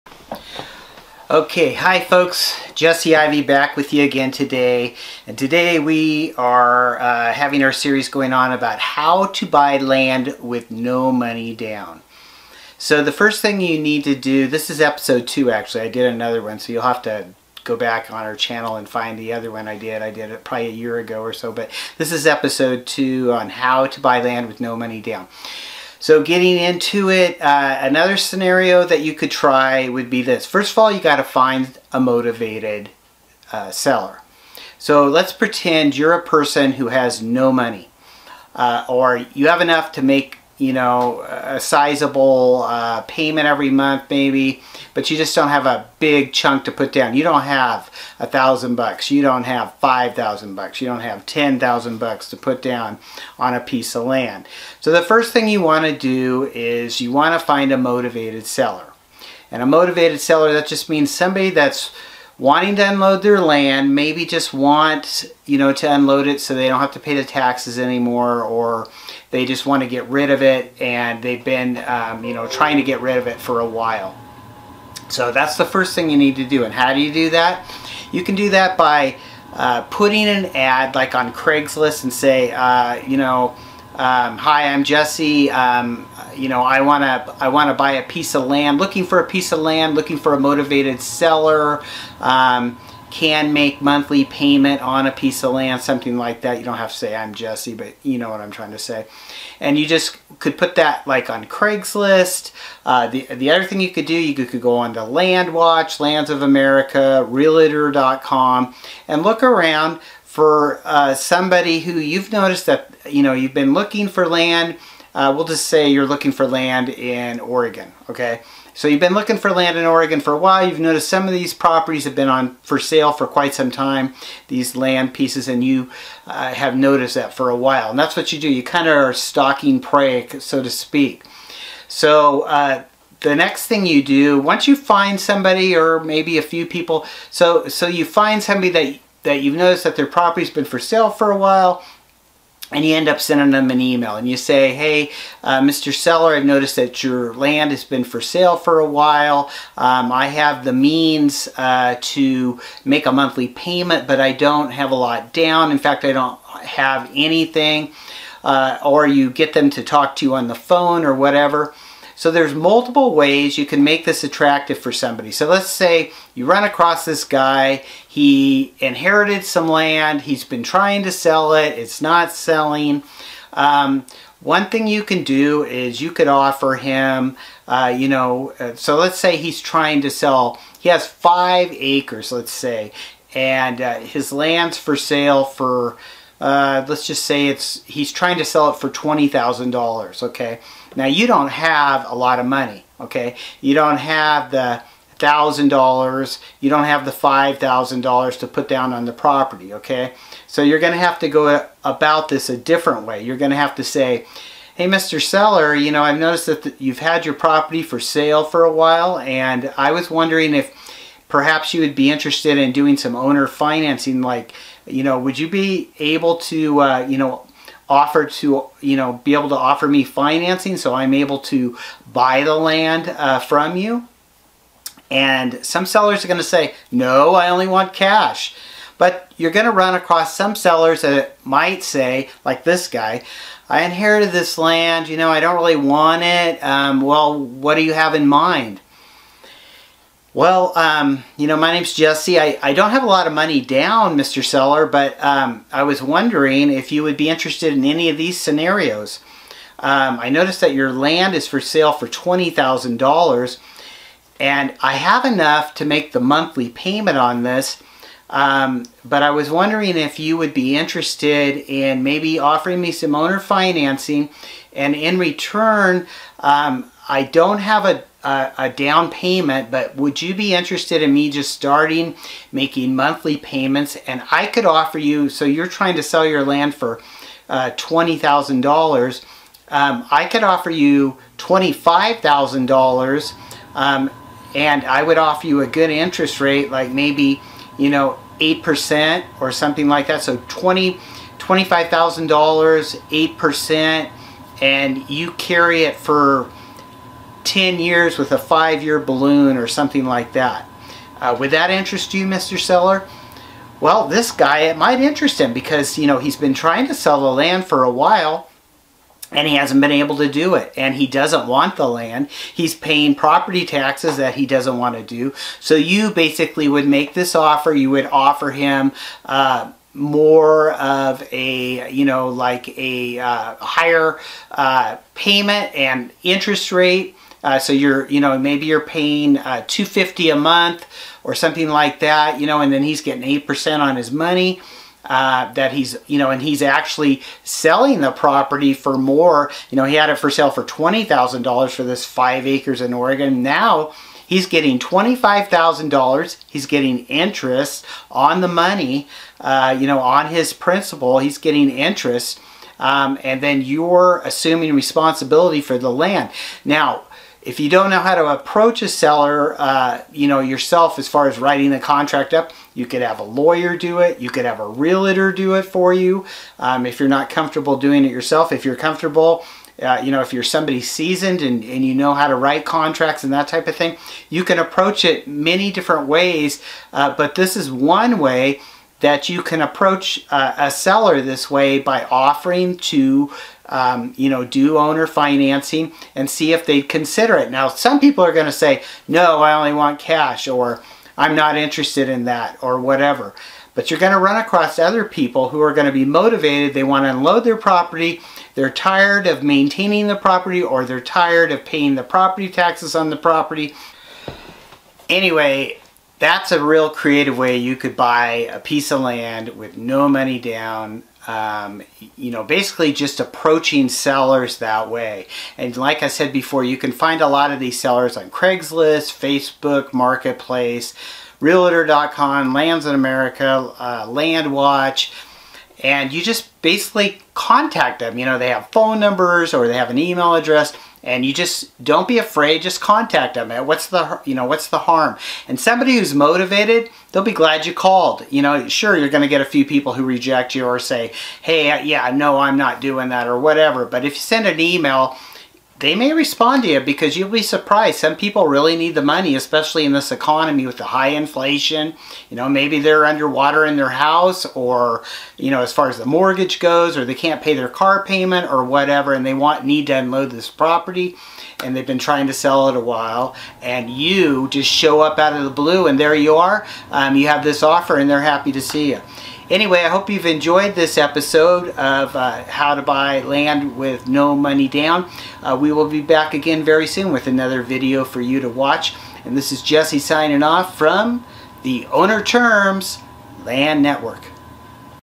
How To Buy Land With No Money Down Ep 2
Hi folks, Jesse Ivy here with our How To Buy Land With No Money Down Ep 2 video. Are you looking to buy land but don't have the money for a down payment? Don't worry, you're not alone. In this video, we'll share with you the insider secrets on how to buy land with no down payment. In this video, we'll walk you through the process of buying land with owner financing. From understanding the benefits to negotiating the terms, you'll be an expert in no time. Whether you're a first-time land buyer or just looking for a more flexible financing option, this video has everything you need to know. So, sit back, relax, and let's get started!

 or  @OWNER FINANCED LAND

[SOCIAL]